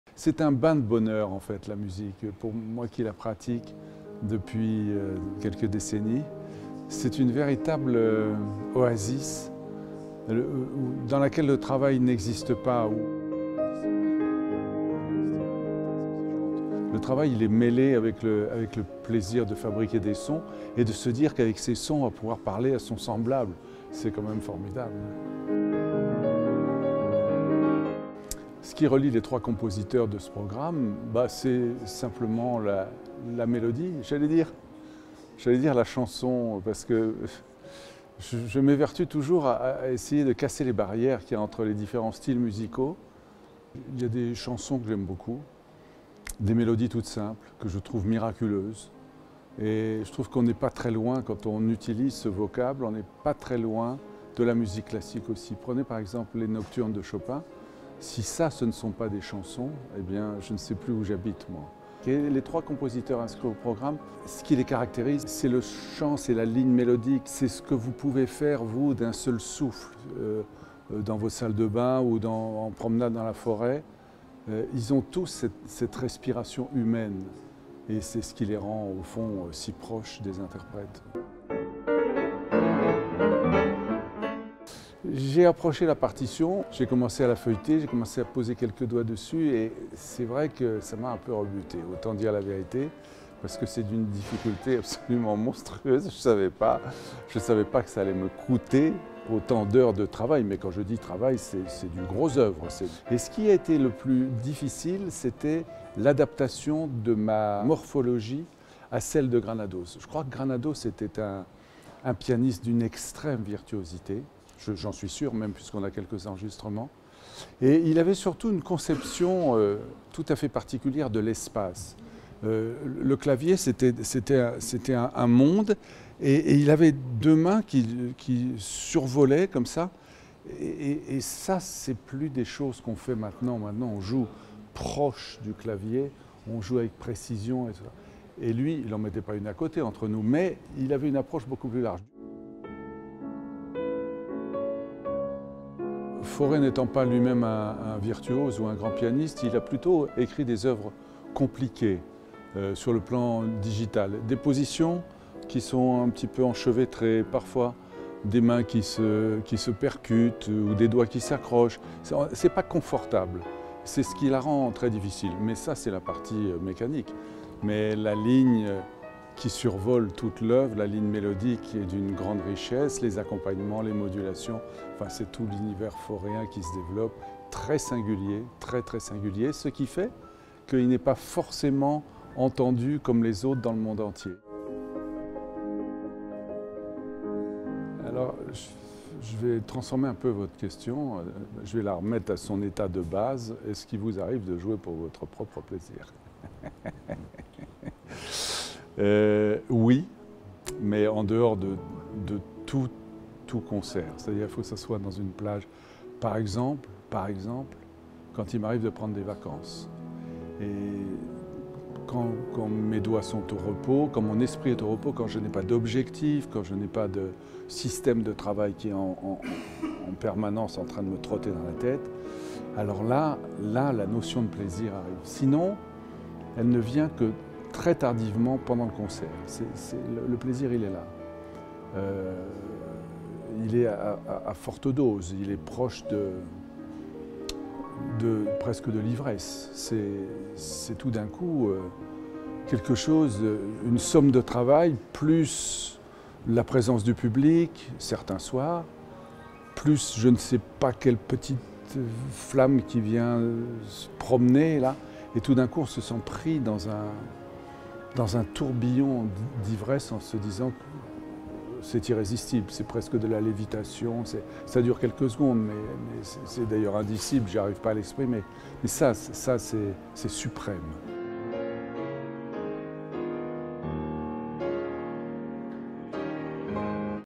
Jean-Philippe Collard : interview au piano [2021]
[INTERVIEW REALISEE POUR SON CONCERT DE 2021]

Le pianiste Jean-Philippe Collard revient au Théâtre des Champs-Elysées jeudi 2 février 2023, pour un programme Fauré, Chopin, Scriabine.

Déroulé de la vidéo
début : Le bonheur qu'apporte la musique au pianiste
00:26 : Le lien entre les 3 artistes [question pour son concert de 2021]